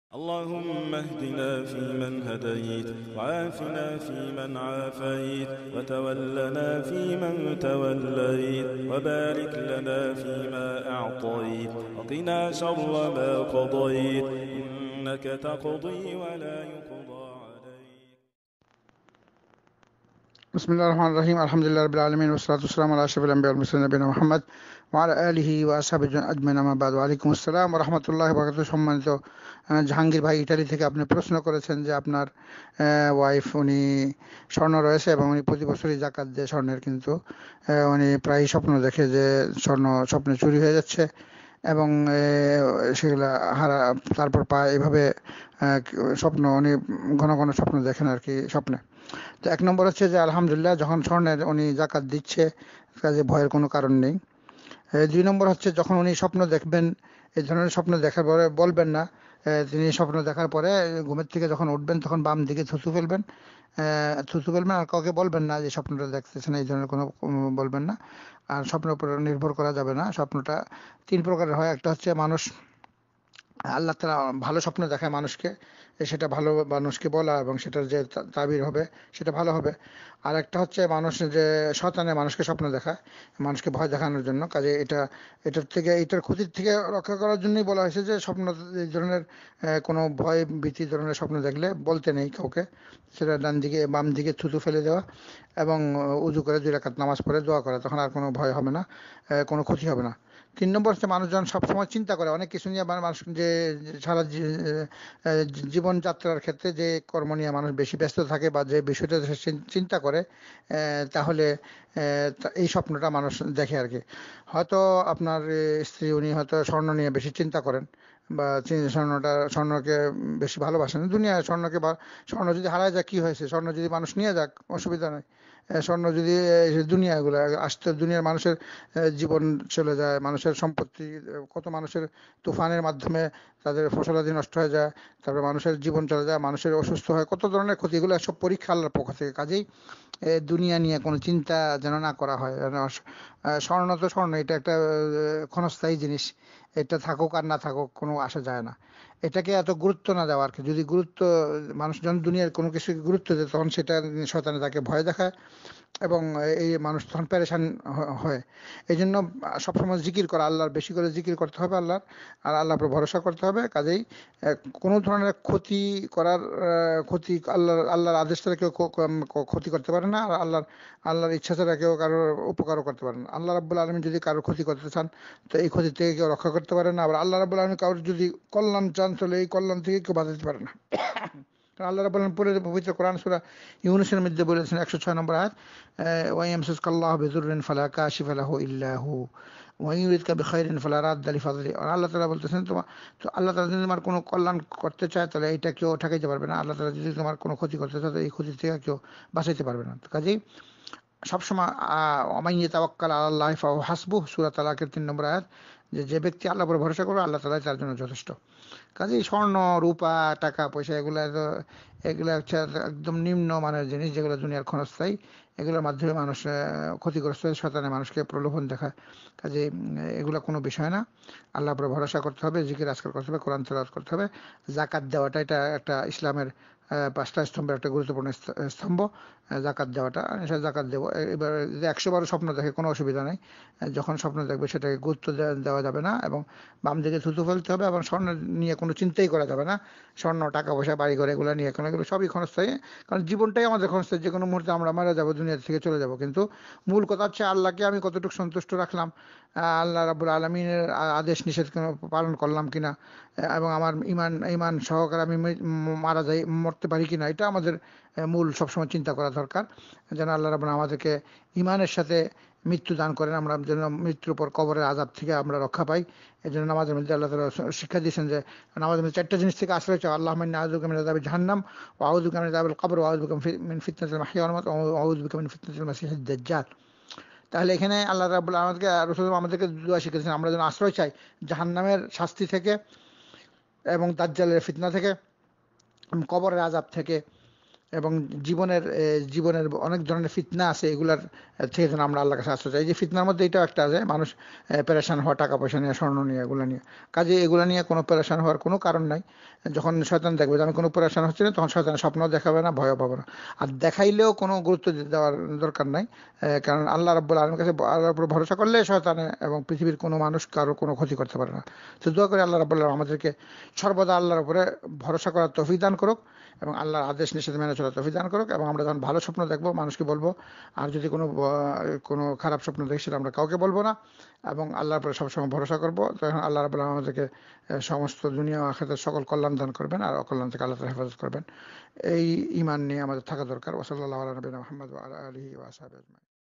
স্বর্ণের যাকাত দেই তারপরেও স্বপনে দেখি স্বর্ণ চুরি হয়ে যাচ্ছে করণীয় কি? শাইখ কাজী জাহিদ মাদানী।
স্বর্ণের যাকাত দেই তারপরেও স্বপনে দেখি স্বর্ণ চুরি হয়ে যাচ্ছে করণীয় কি? শাইখ কাজী জাহিদ মাদানী। স্বাগতম আপনাকে AL FURQAN media.